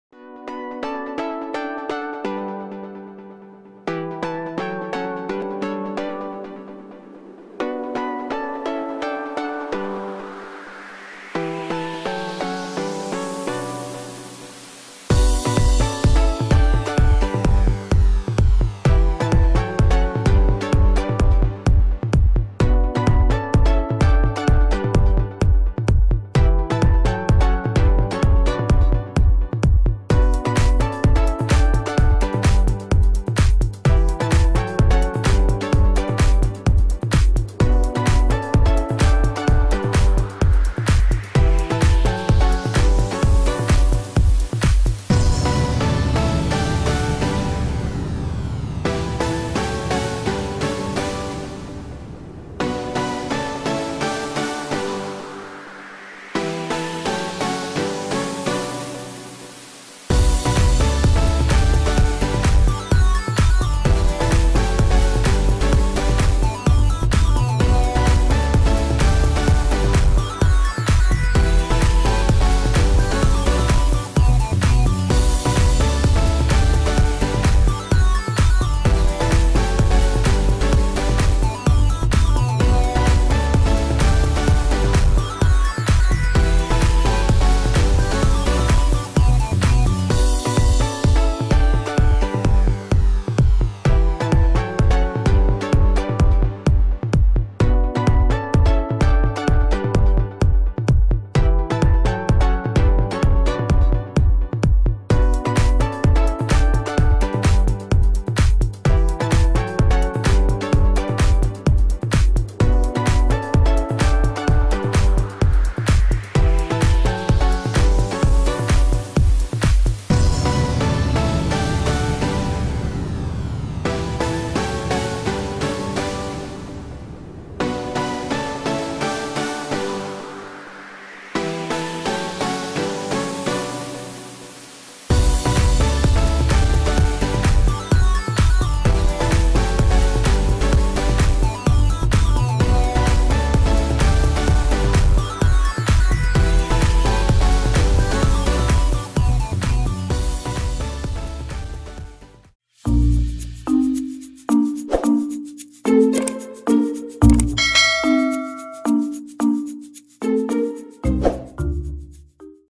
Milk Kova | Sweet recipes | Paal Kova | Mamma's Kitchen
Hi guys, Welcome to Mamma’s Kitchen…..
In this video we are going to make a milk sweet, which is milk kova (palkova). It is a tasty sweet recipe that everyone can try at home easily. But it requires about 40 to 50 mins to prepare it.

Also follow us on various social media's